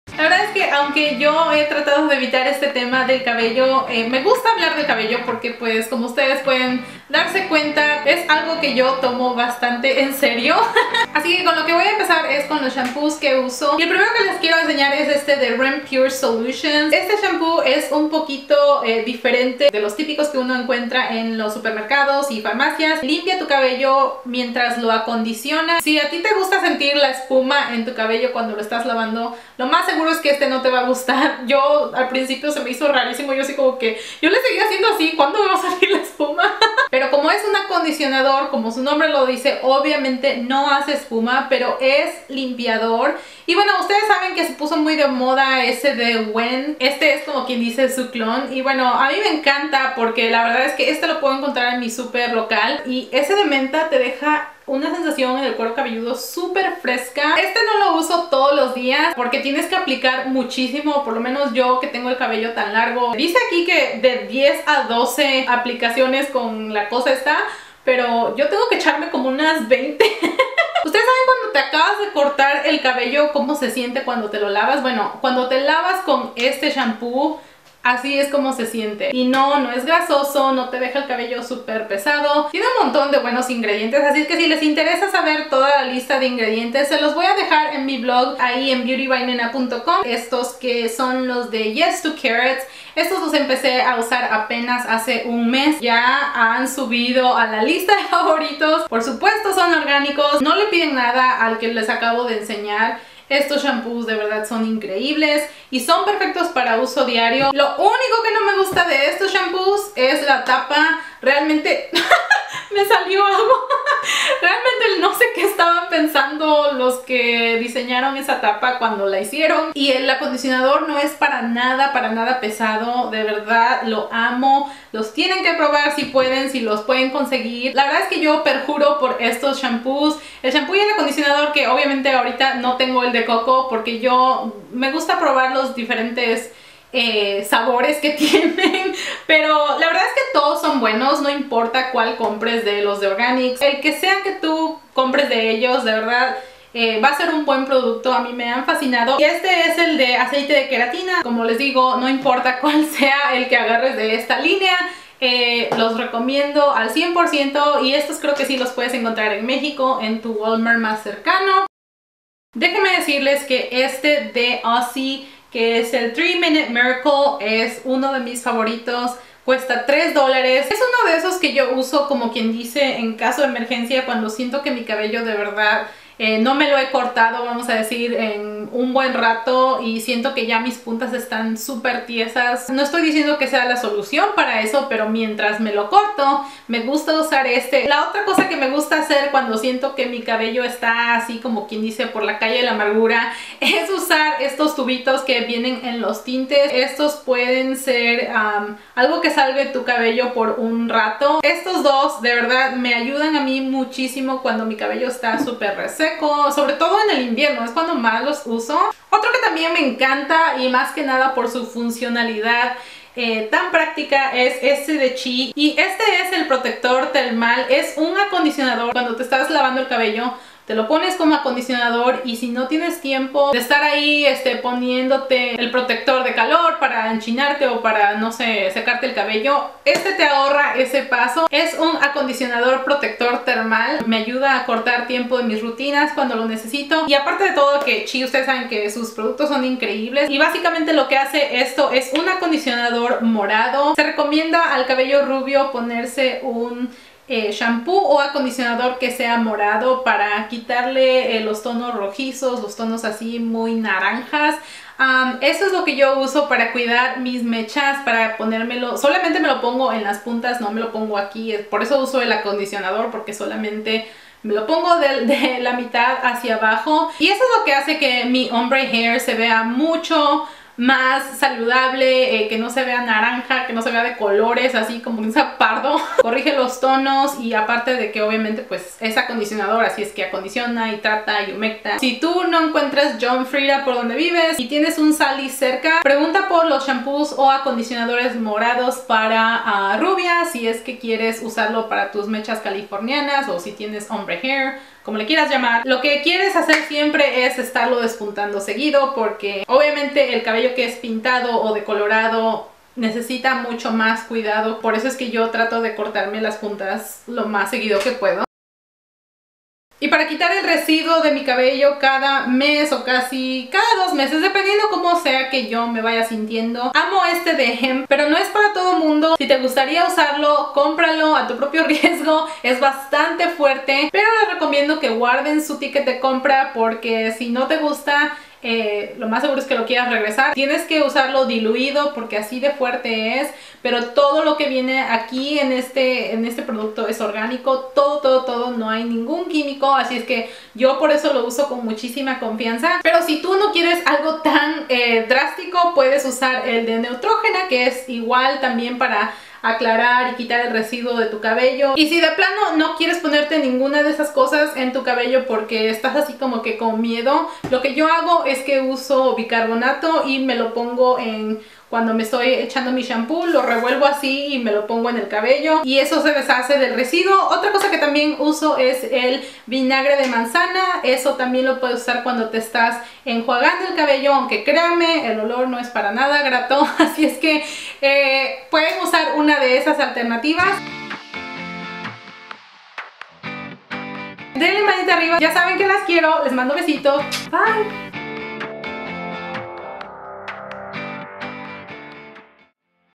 Productos para el cabello seco y maltratado ♥BeautybyNena
En este video les muestro como cuido mi cabello (mechas californianas/ombre hair)

sigueme!
❤INSTAGRAM. @beautybynena